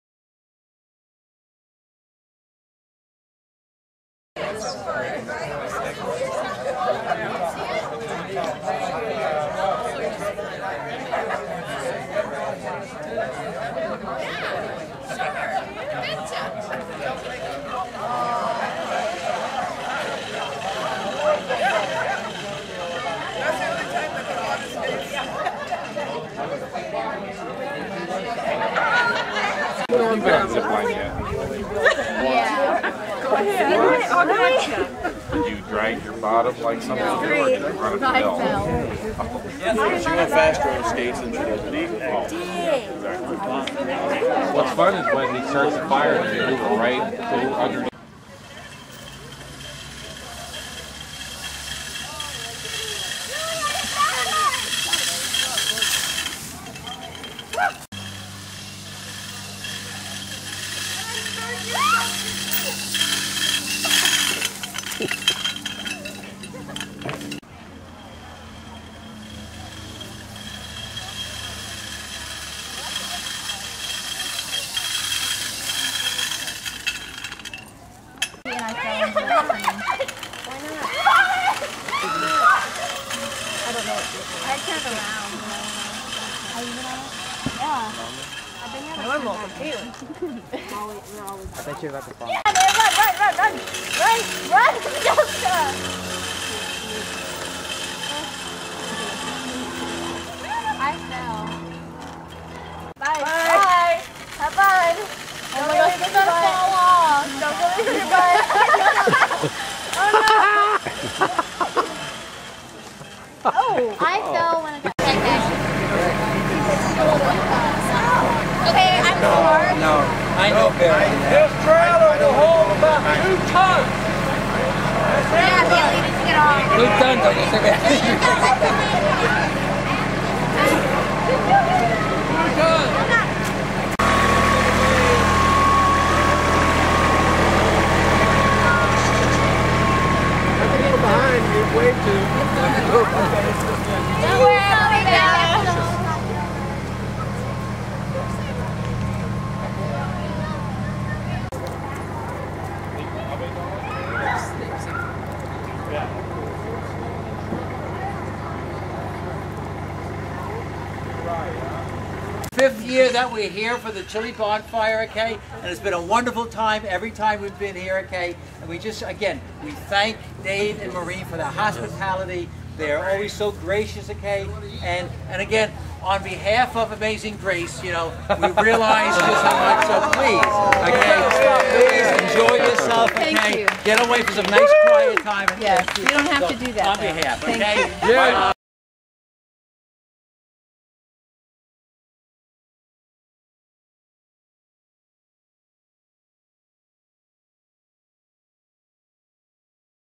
Amazing Grace 2014 Chili Bonfire Cookout
Amazing Grace Lutheran Church held its 5th annual Cookout on January 26, 2014.  Everyone enjoyed the delicious food, and many participated on the hayrides and zip line.